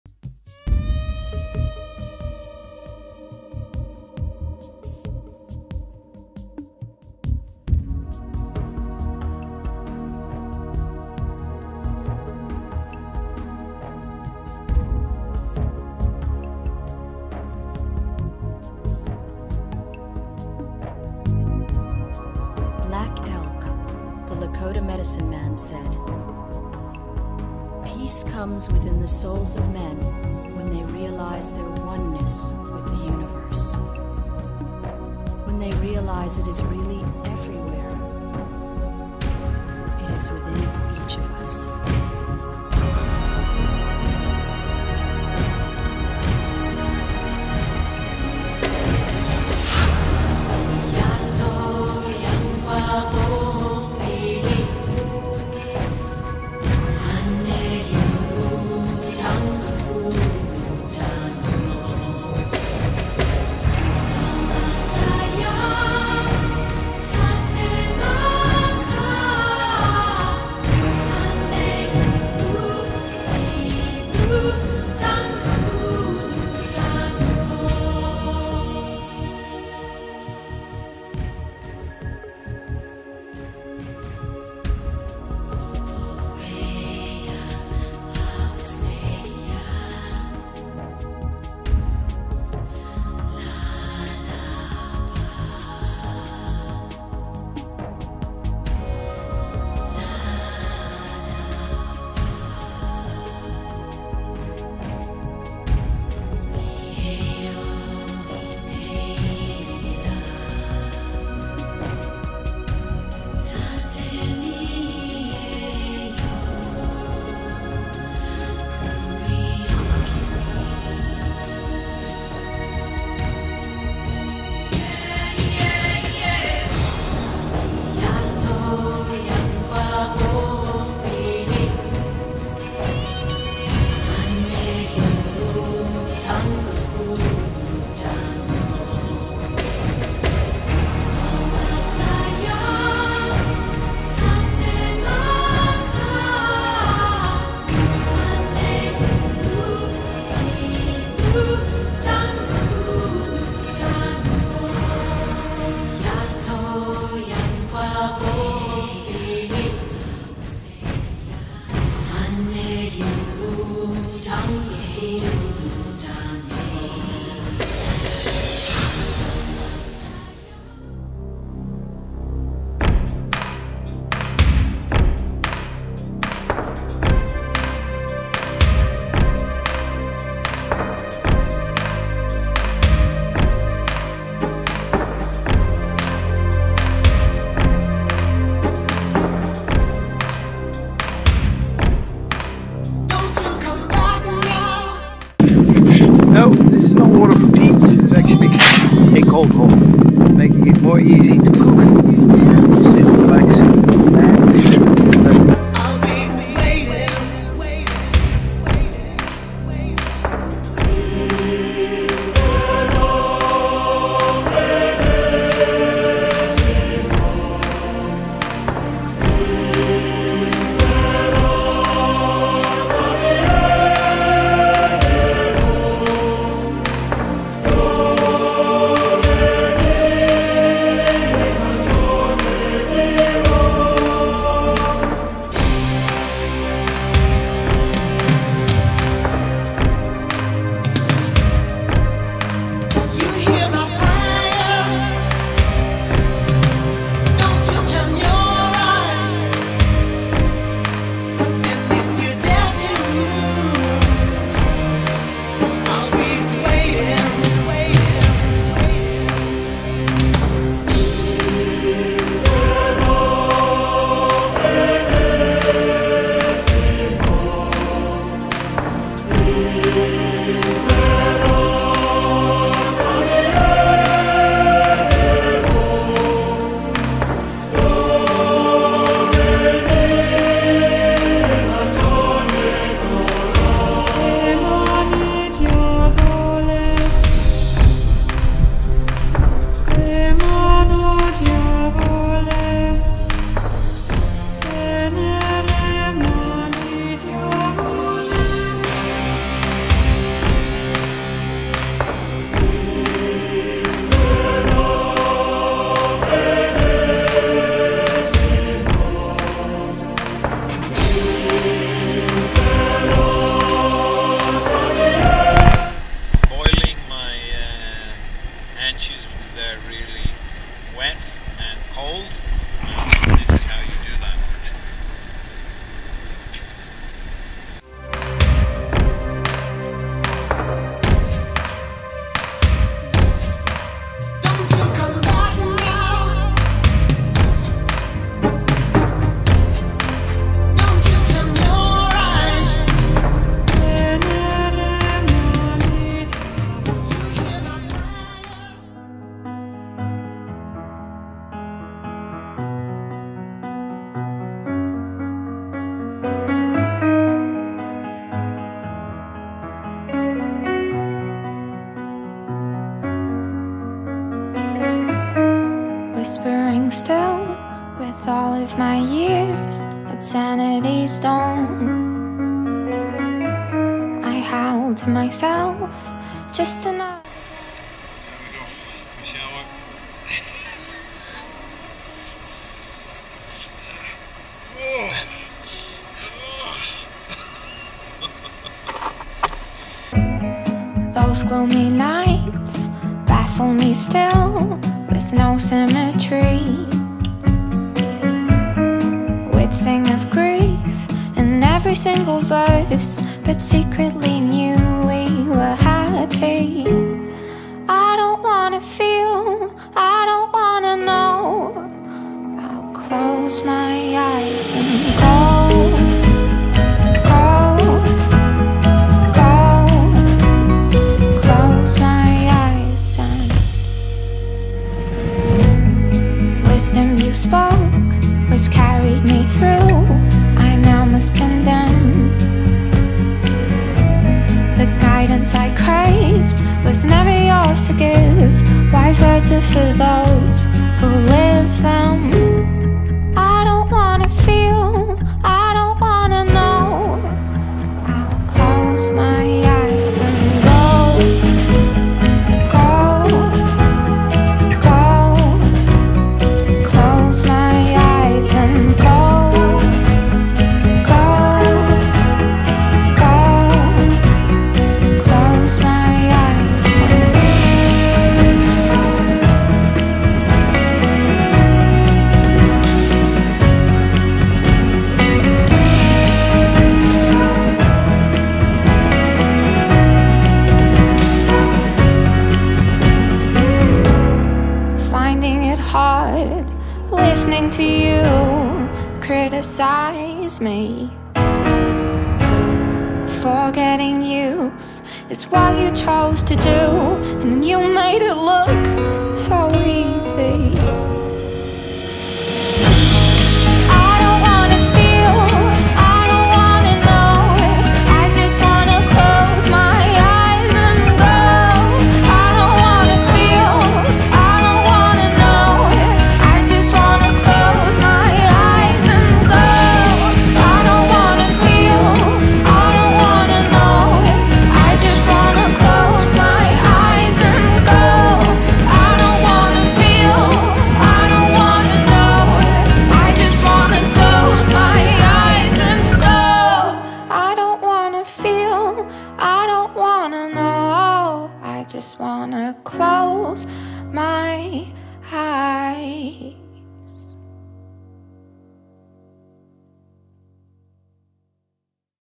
Arctic voyage 4K
A trip around the arctic circle to vind some good snow-kite places using dog sledges as transportation, while sleeping in tents.